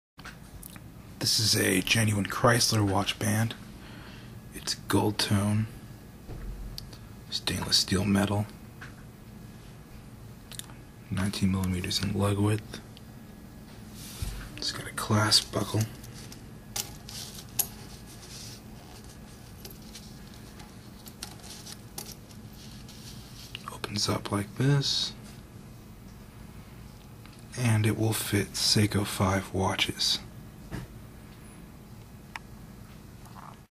Genuine Kreisler Gold Tone Stainless Steel 19mm Clasp Band
Genuine Kreisler, 19mm, Gold Tone, Stainless Steel Metal, Regular Length, Clasp Buckle.

Model No. AU05277N.